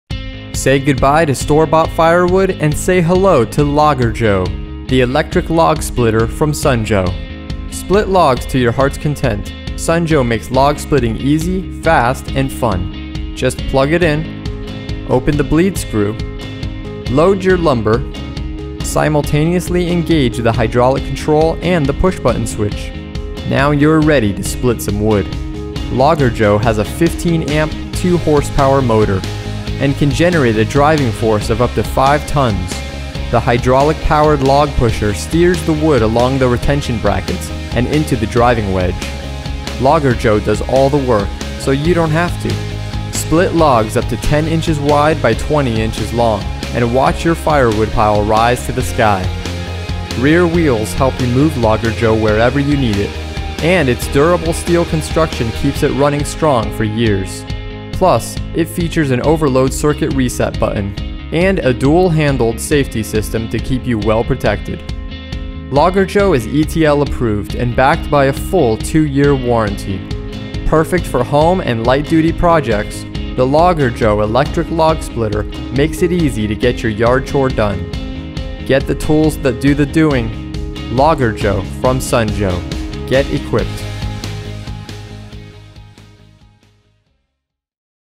LJ601E - Sun Joe Logger Joe 15 AMP 5 Ton Electric Log Splitter - Live Demo
The Sun Joe Logger Joe LJ601E electric log splitter is a portable log splitter made for home or light-duty use. Its powerful 15-amp/2-horsepower motor generates an approximate driving force of 5 tons, which can easily split your standard size firewood log measuring 10 inches by 20 inches. Equipped with rear wheels, the Logger Joe electric log cutter can be easily moved to different work areas. Plus, it's durable steel construction ensures long-lasting wear. This electric log splitter offers several safety features including including an overload circuit reset button and a dual-handed safety switch system. This machine is ETL-inspected and approved and comes with a full two-year warranty from the manufacturer.